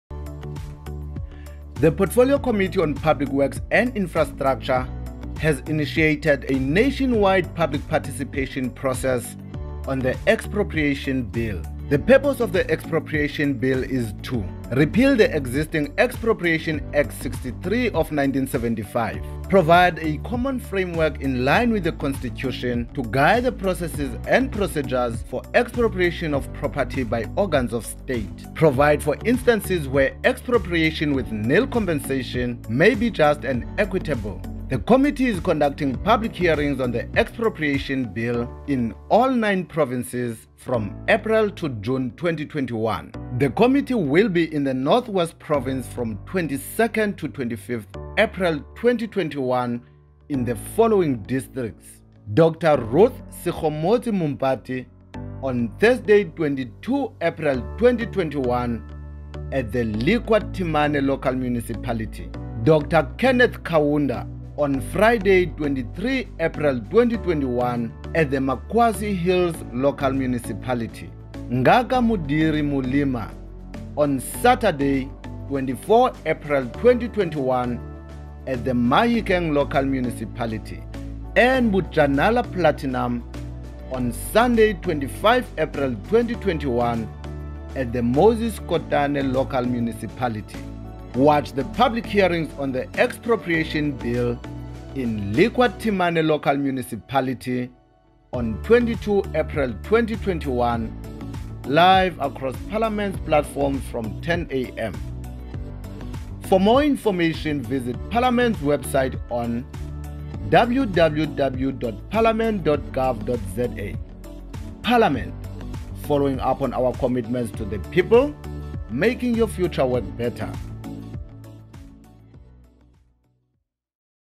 EXPROPRIATION BILL PUBLIC HEARINGS HEAD TO THE NORTH WEST, 21 April 2021
The Portfolio Committee on Public Works and Infrastructure will from tomorrow, Thursday 22 April 2021, until Sunday 25 April 2021, hold public hearings on the Expropriation Bill [B23-2020] in the North West province. The first hearings will be held at the Christiana Town Hall, in Christiana.

The purpose of the Expropriation Bill is to repeal the existing Expropriation Act of 1975 to provide a common framework in line with the Constitution, to guide the processes and procedures for the expropriation of property by organs of state, and to provide for certain instances where expropriation with nil compensation may be appropriate in the public interest.

In line with Parliament’s strategic objective of enhancing access to the work of Parliament, legislative process and improving public participation in general, the committee resolved to visit all provinces to garner public views on the proposed legislation. The intention of the public hearings is to enrich the Bill and to ensure that the Bill is responsive to the views and needs of individuals and groups on the ground.

Similarly, the committee decided to hold public hearings during both weekdays and weekends to afford more participation of individuals and stakeholders as possible.

The committee is mindful that the scheduled public hearings happen during the Adjusted Alert Level 1 lockdown regulations, and has taken due regard to ensure adherence to Covid-19 guidelines and regulations.